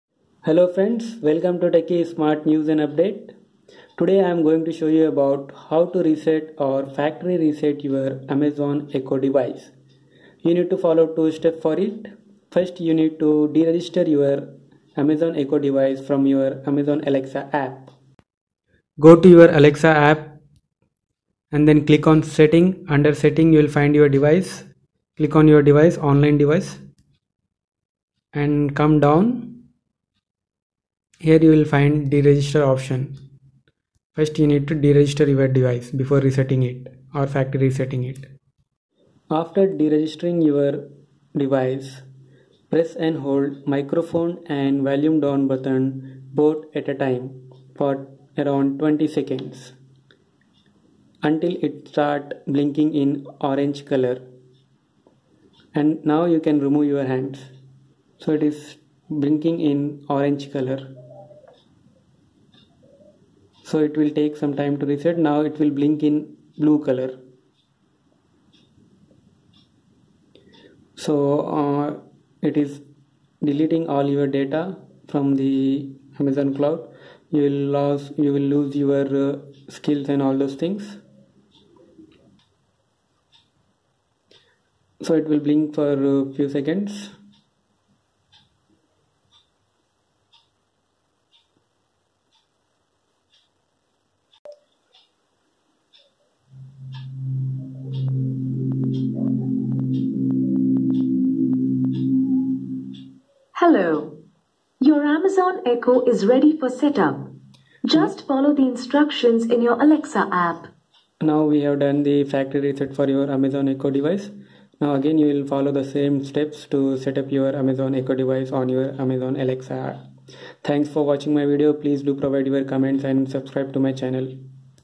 How to Factory Reset Amazon Echo
Hello, Friends Welcome to Techy Smart News...

 In this video, I will show you how to de-register and factory reset your amazon echo/amazon echo dot.
00:00 - Intro
00:18 - How to Deregister Device
00:39 - Reset
01:45 - Rest Completed